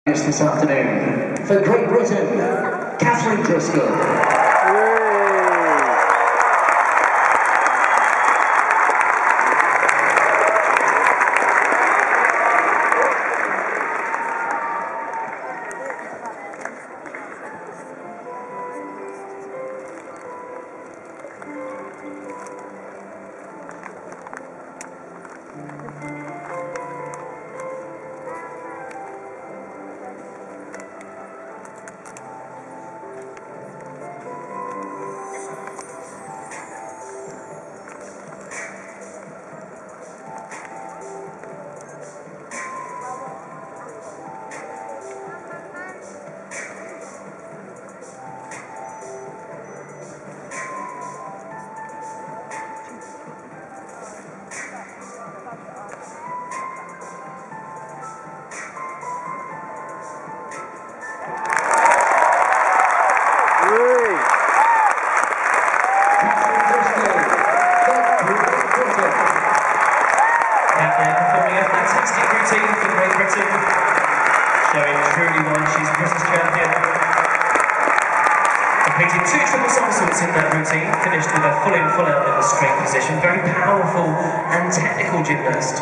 Kat Driscoll - Trampoline Finals - London Prepares Olympic Event
Rooksy & Holly at the O2 seeing Kat Driscoll GB placed 4th